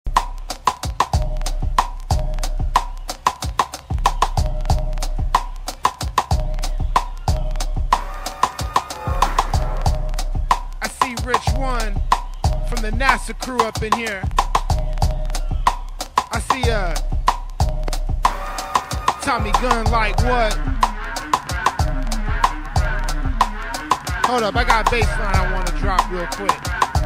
Trump Toilet Paper Message Predictive Programming Mockery (Trump Flu)
In 2018 President Trump Sent America A Mocking Message About Toilet Paper. It Appears Staged How He Walks Up The Steps And Then Deliberately Wipes The Toilet Paper Off His Shoe. Donald Trump's Buddies, The Koch Brothers, Are Actually Real-Life Toilet Paper Czars.